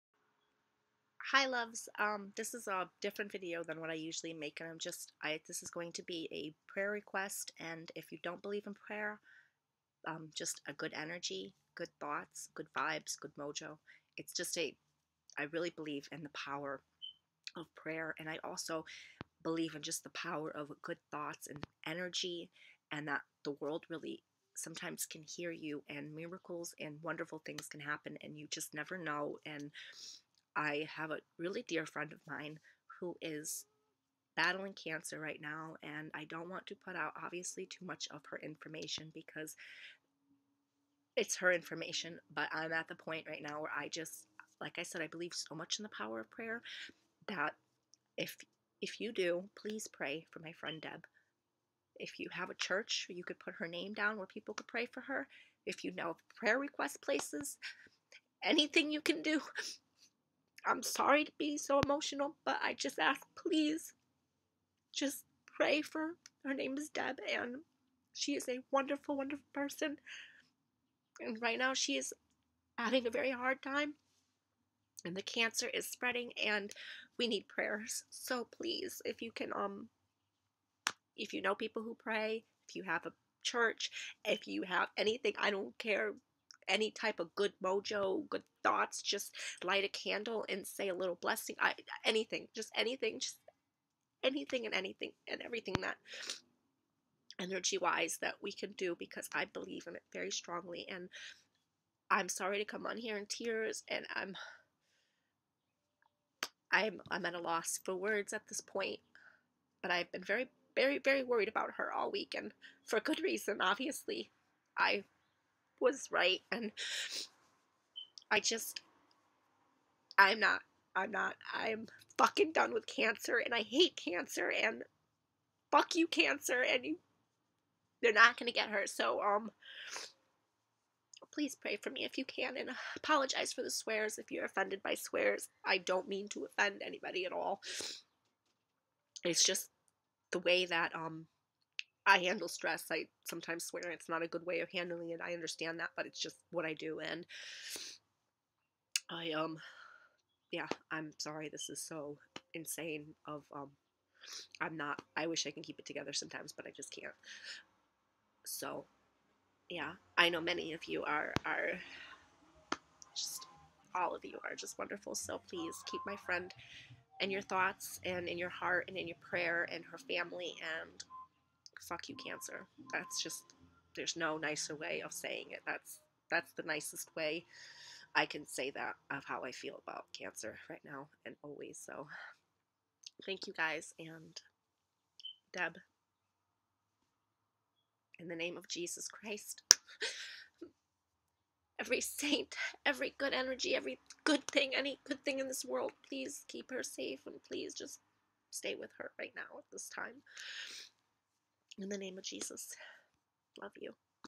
Prayer Request. FUCancer!
Sadly my friend passed on May 2nd, Please keep her family in your prayers and thoughts, Thank you

Please Pray or send good thoughts and energy to my good friend Deb, She is fighting for her life, she is a wonderful person, mother and friend and I ask for any type of prayers or good thoughts to be sent her way, I truly believe in pray and energy, I believe in miracles and I have strong faith. It is strong and the more people at it the stronger it becomes, Please help. I truly appreciate it